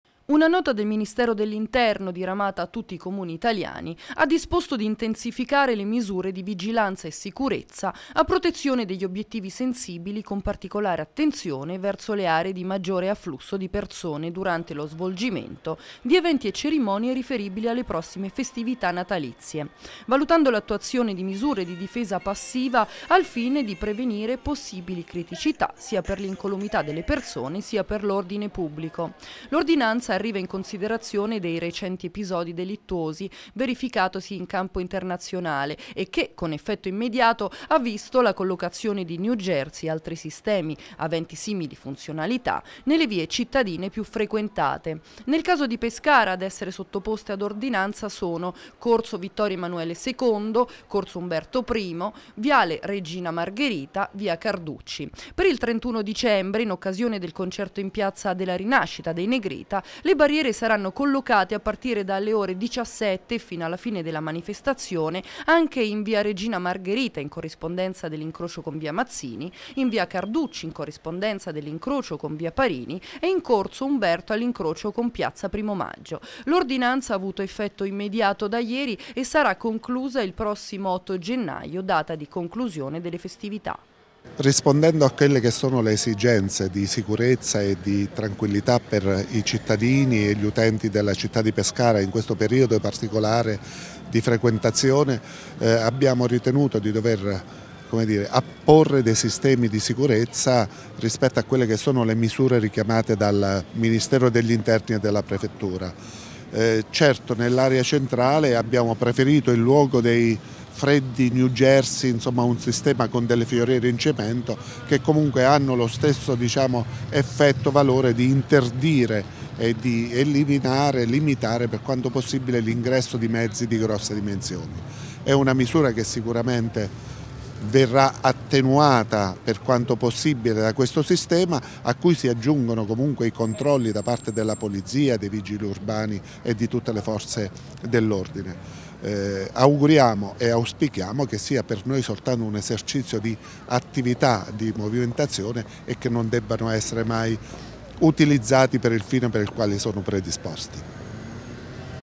Pescara. Misure antiterrorismo per le festività
Con una nota del Ministero dell'Interno diffusa a tutti i comuni d’Italia, sono state disposte misure di sicurezza passiva anti terrorismo in tutti i luoghi con particolare affluenza di persone.
Il comune di Pescara ha già rese note tutte le vie interessate è messo in opera le precauzioni richieste.